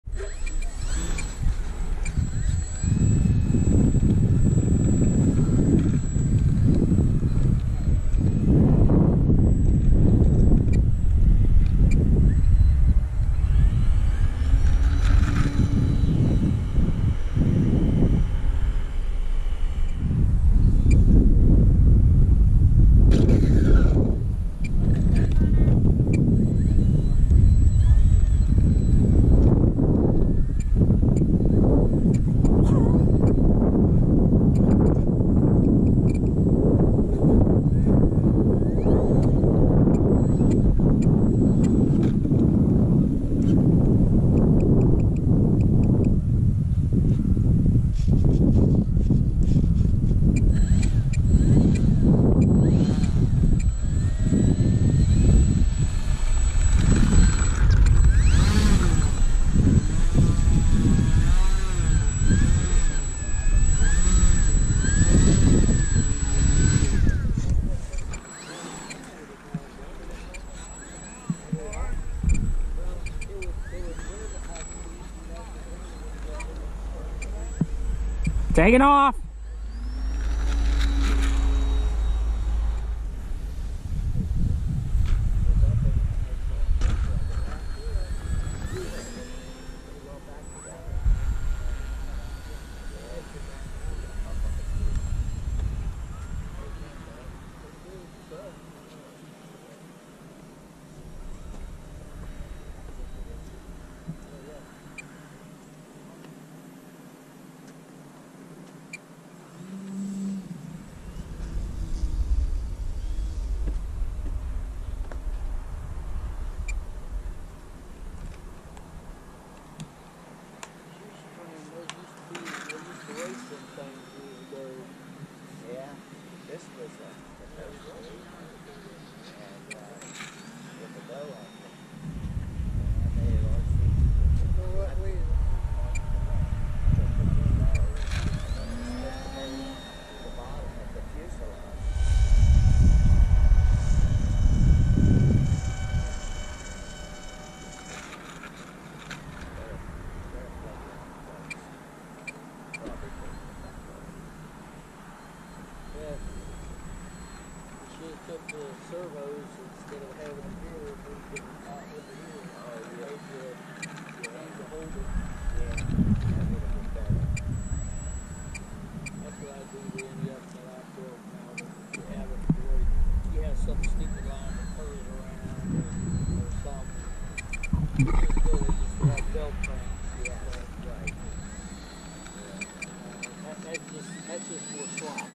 Day at the field with my Stinson
It was two different days but it was fun flying the RC Stinson Reliant.